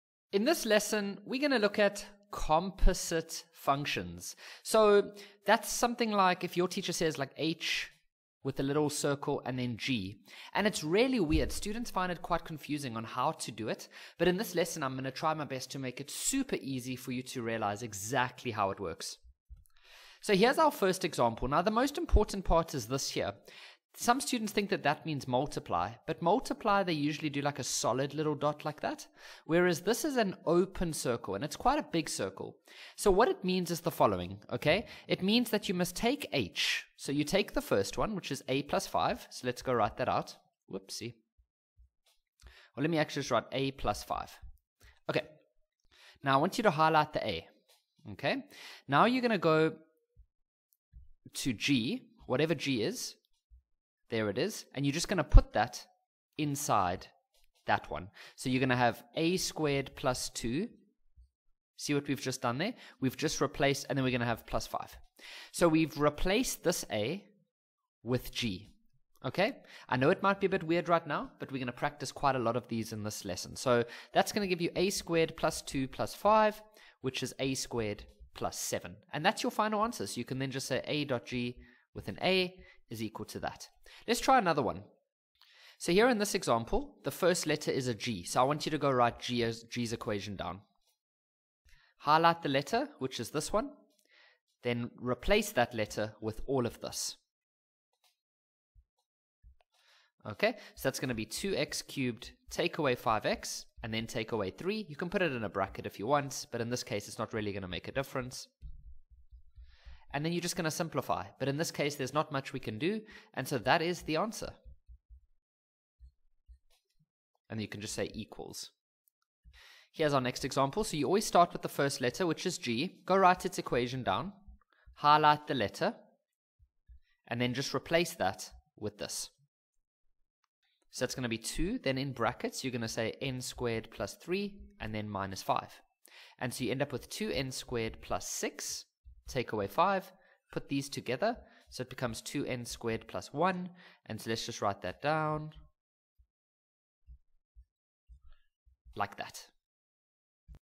Composite Function Algebra 2
This algebra video teaches composite functions. It is ideal for learners doing algebra in the American High School Curriculum but is also suited to all other learners